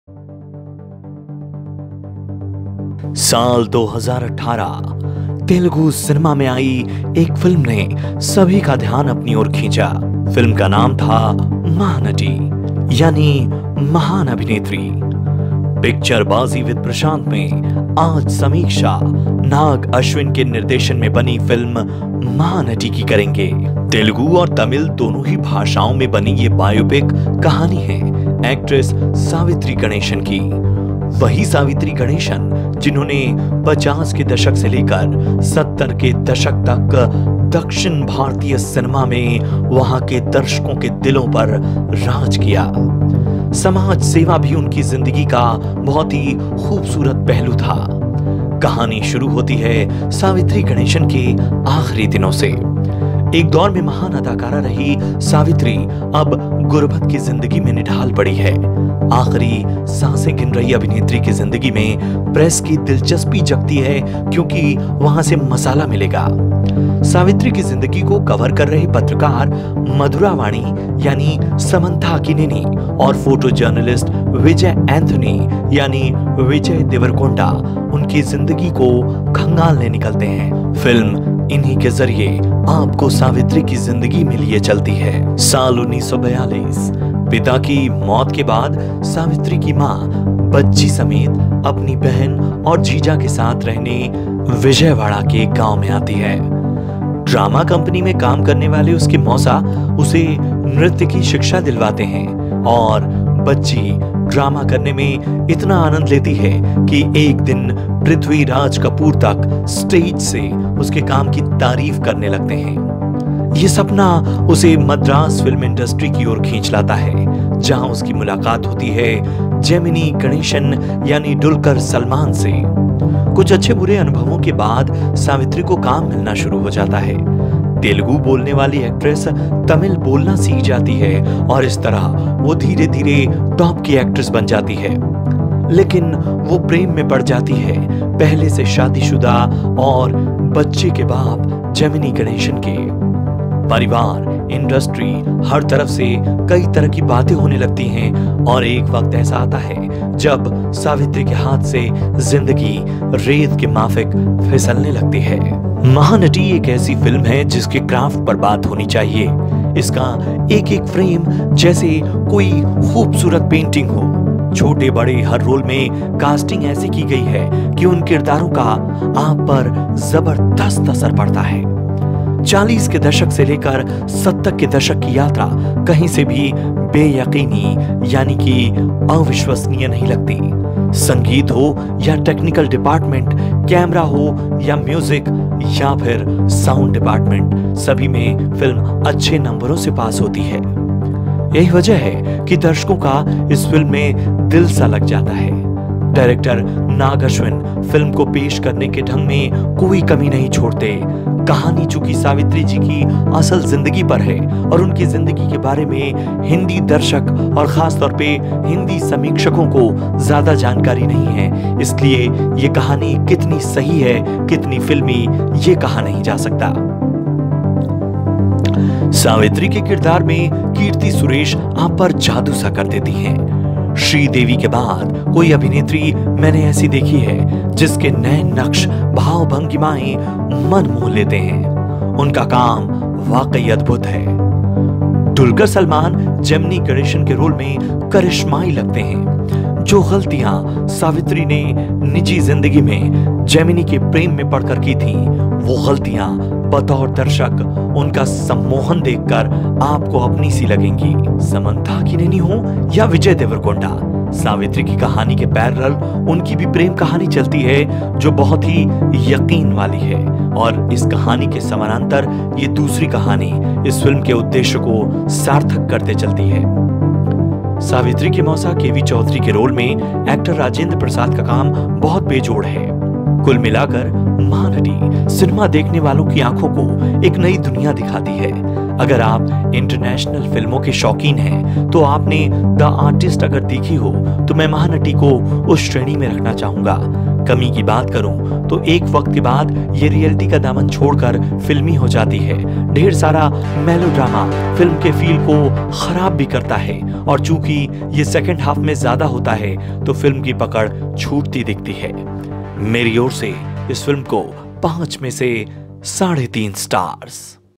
Telugu Masterpiece Mahanati Podcast Review | Keerthy Suresh | Dulquer
𝗠𝗮𝗵𝗮𝗻𝗮𝘁𝗶 𝗠𝗼𝘃𝗶𝗲 𝗔𝗻𝗮𝗹𝘆𝘀𝗶𝘀 Podcast By Prashant Pandey